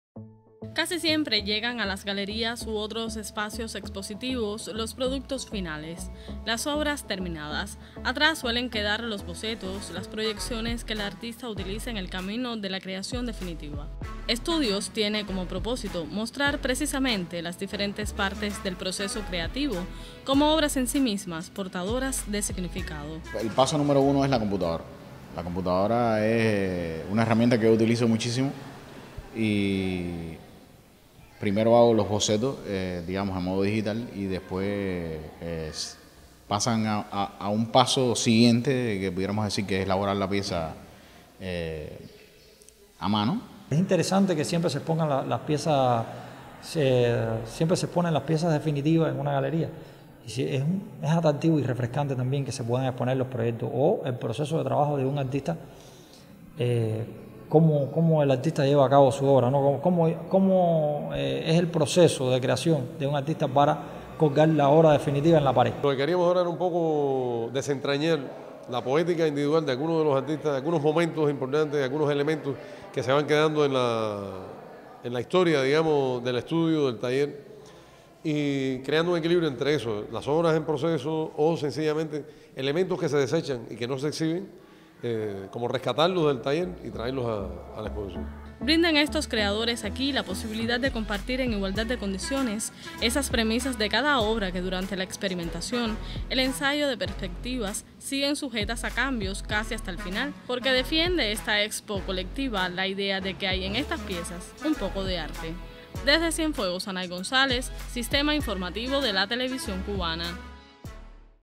Inauguran en Cienfuegos exposición de artes visuales “Estudios”
La galería Mateo Torriente de la Unión de Escritores y artistas de Cuba en Cienfuegos, acoge por estos días una nueva muestra colectiva. La Expo lleva por nombre “Estudios” y agrupa a creadores locales de reconocida trayectoria.